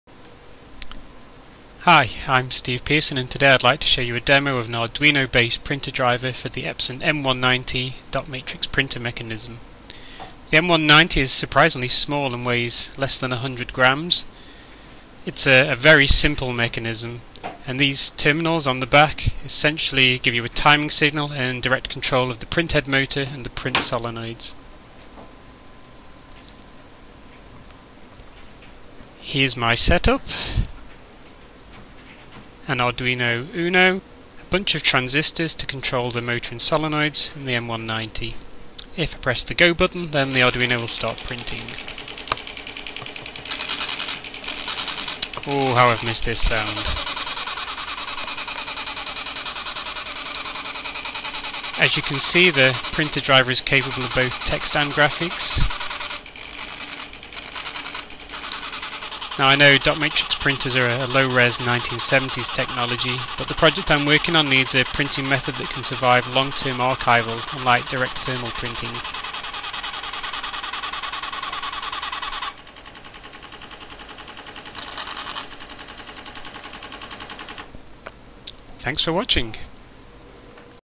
Arduino Proejct : Connect Epson M-190 Printer
This demo shows an Arduino UNO controlling an Epson M-190 Printer Mechanism.

The printer driver is capable of printing both text and graphics.

Source code available here.